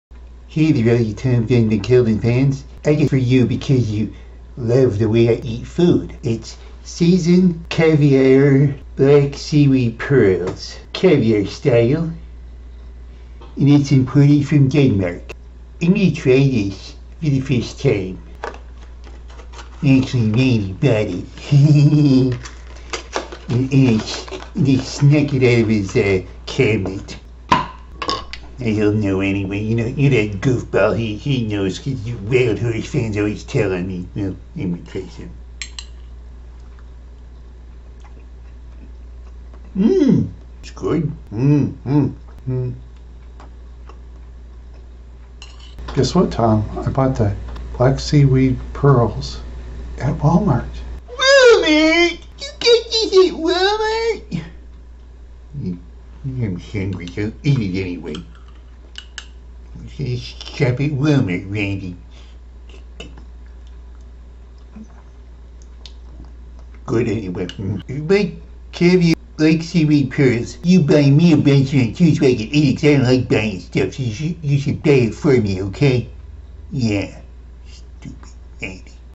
Review of Caviart black seaweed pearls caviar style imported from Denmark by Tom Ven Mc Helden.
Rich great taste plant based sustainable seaweed, and cholesteroI free. I wish the title can fit the words Maple Grove but Randy doesn't tell YouTube anything!  Stupid Randy!